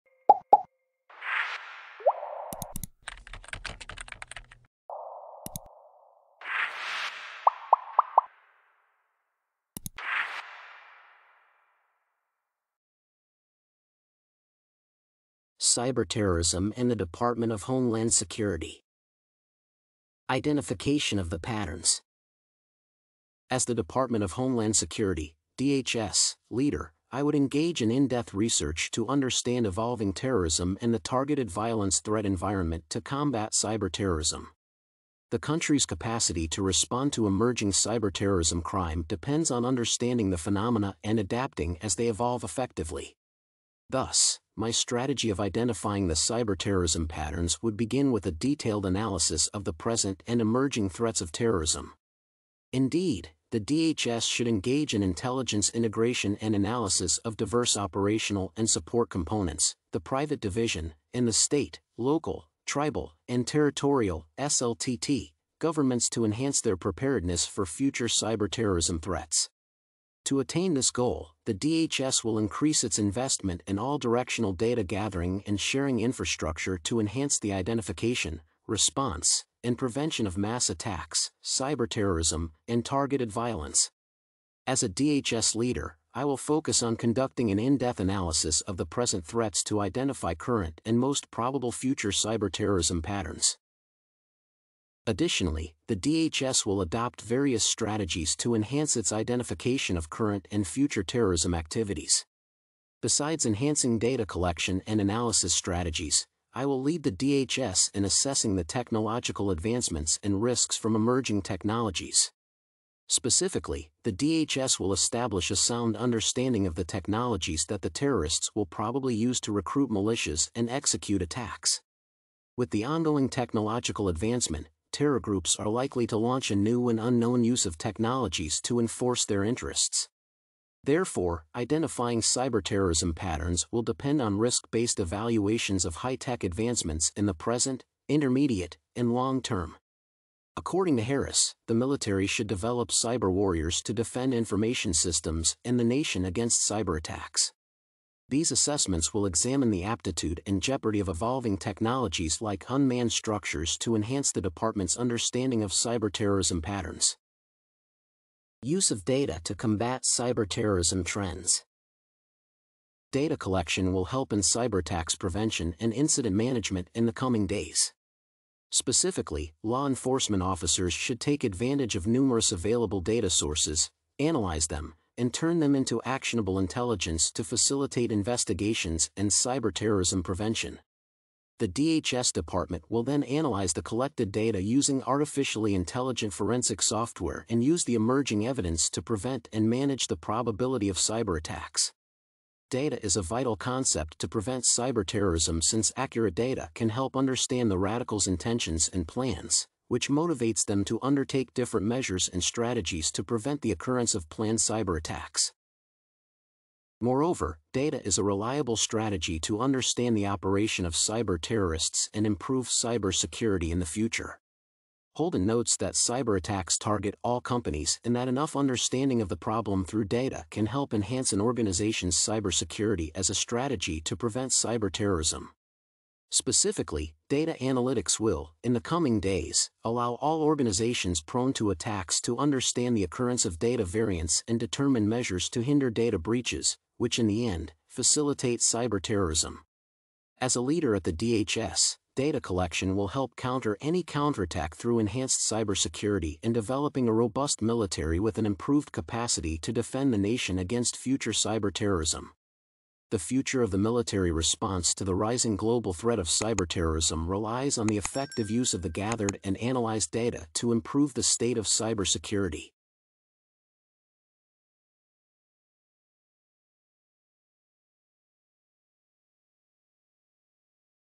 Cyberterrorism and the Department of Homeland Security - Essay Example
📝 Essay description: The Department of Homeland Security leader should engage in research to understand evolving terrorism and the targeted violence threat environment to combat cyberterrorism.

ℹ️ Subjects: Anti-terrorism Legislation
ℹ️ Words: 564
ℹ️ Pages: 2

📚 Reference list:
Department of Homeland Security (2019). Department of Homeland Security Strategic Framework for countering Terrorism and Targeted Violence. The US Department of Homeland Security
Harress, C. (2014). Obama says cyberterrorism is the country’s biggest threat, US Government Assembles ‘Cyber Warriors.’ International Business Times, 18.
Holden, D. (2015). Is cyber-terrorism the new normal? Wired. Web.